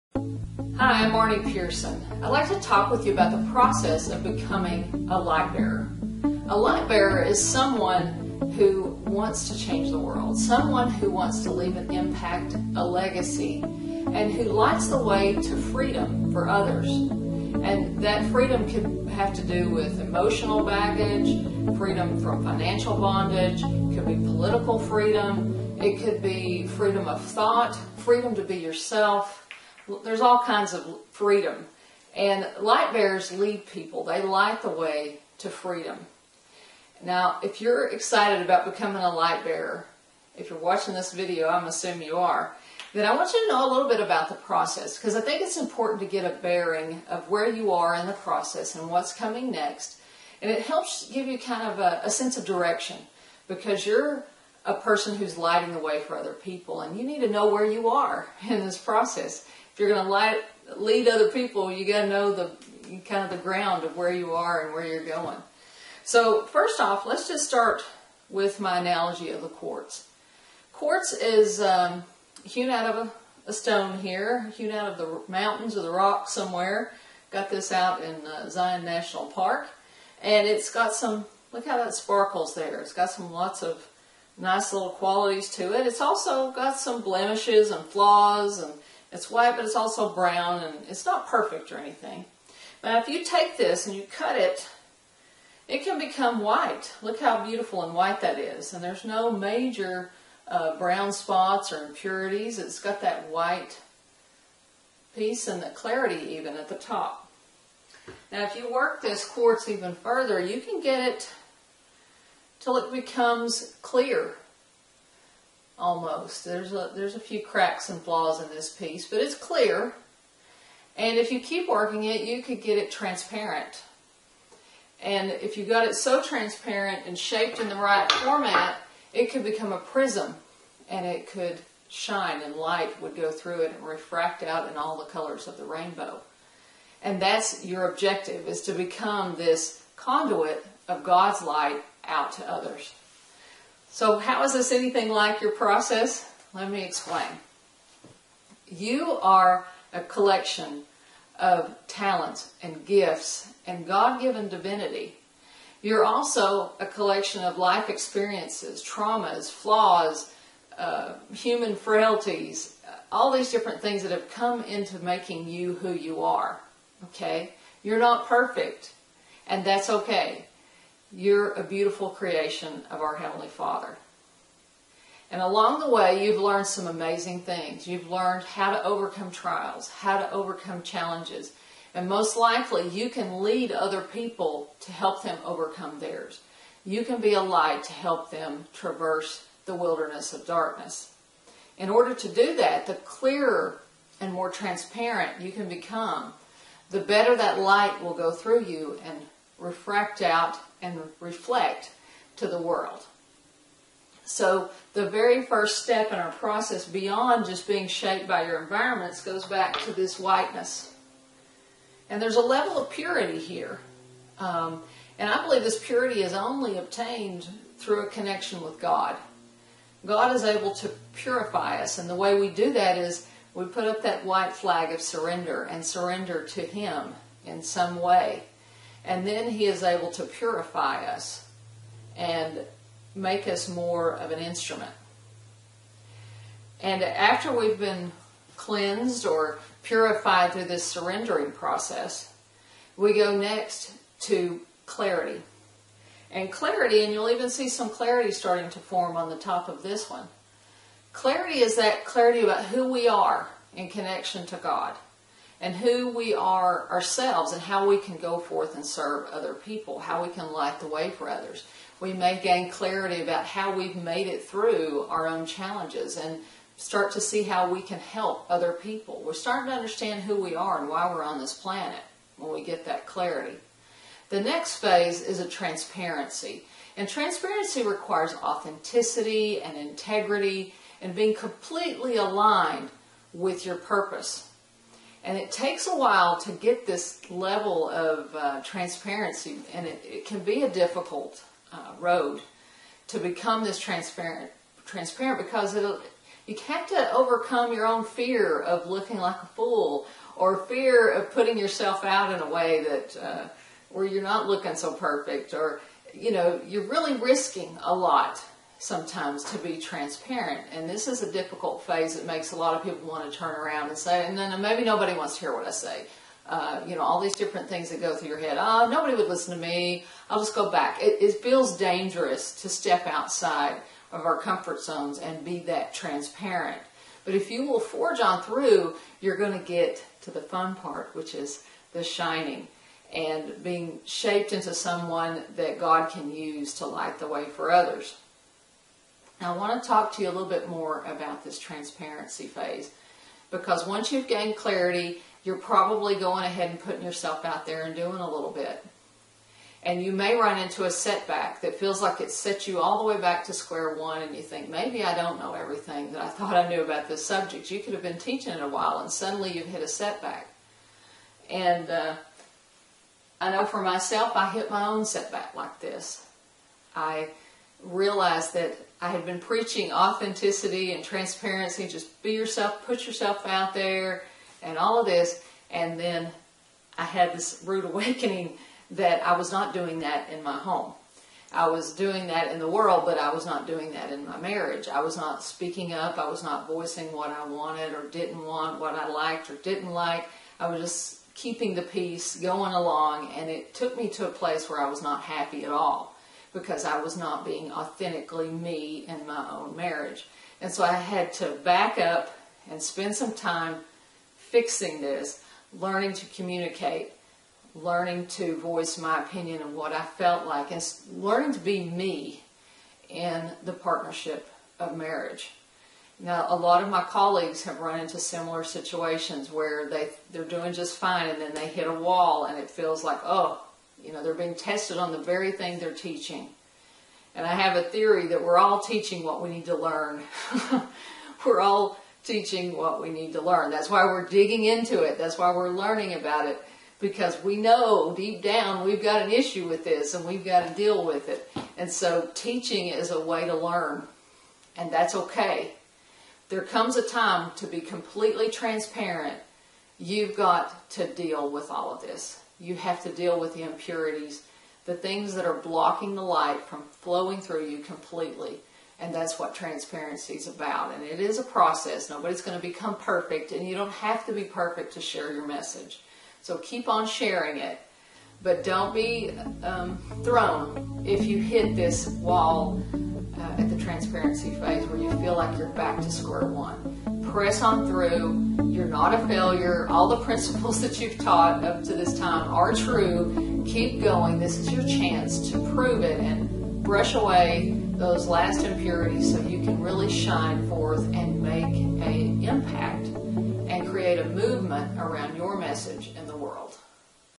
Light the World Cycle: Why Sometimes You Feel Like You're Back to Square One
Marnie Pehrson outlines the process one goes through to light the way for others and impact the world with your message. A look at the phases of development and why sometimes we can feel like we're back to square one.